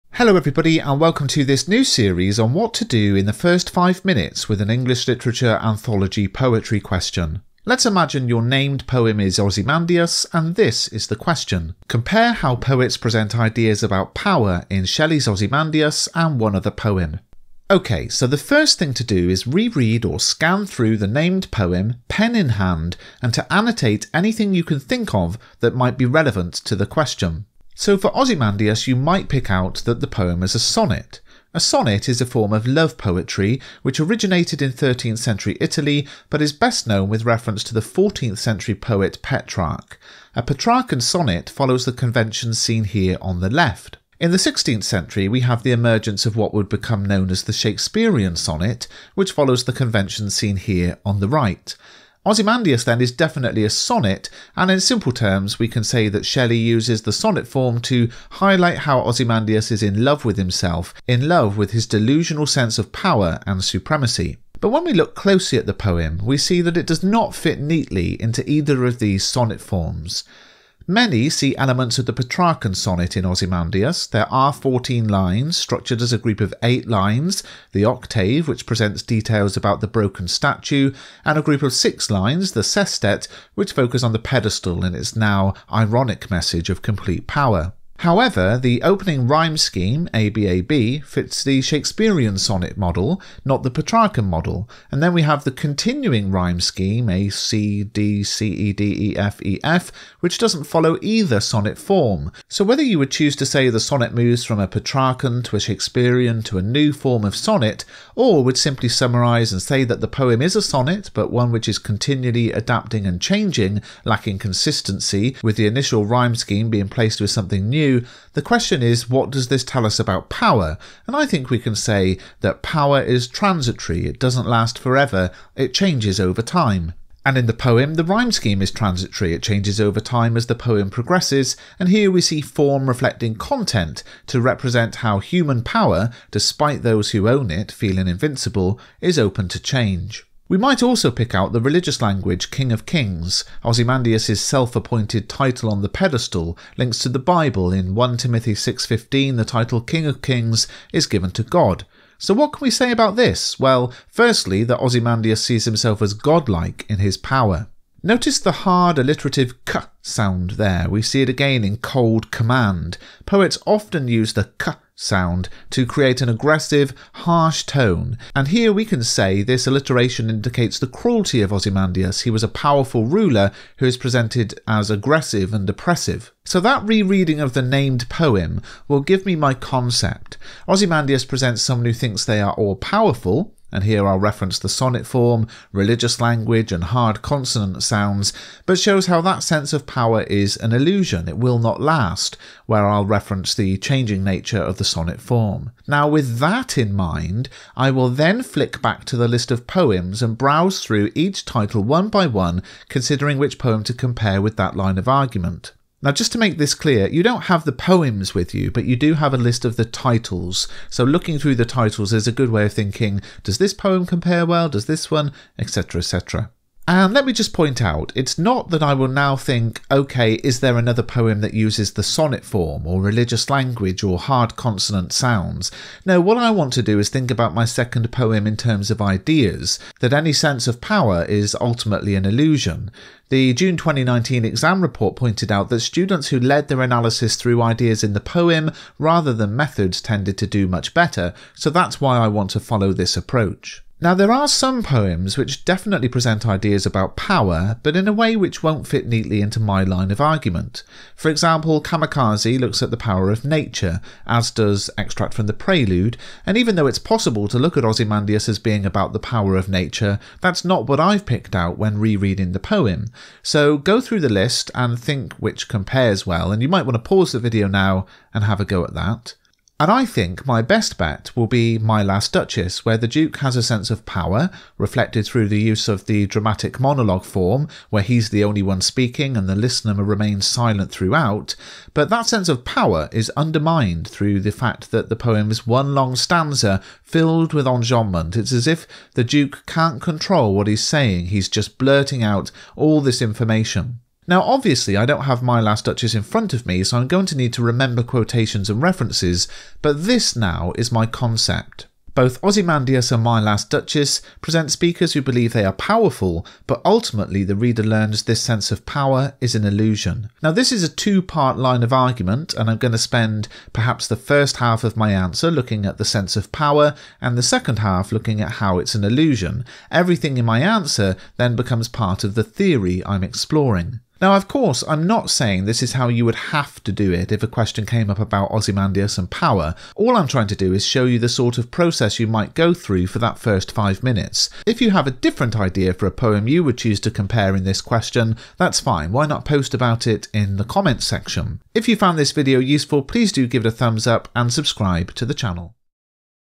How to Spend the First Five Minutes: Anthology Poetry Exam Question (Ozymandias as Named Poem)
How I would spend the first five minutes of the exam if the anthology poetry question was based on the presentation of power in 'Ozymandias'. You can apply the same approach to every poem and topic.

Buy my revision guides in paperback on Amazon*:

AQA English Language Paper 1 Practice Papers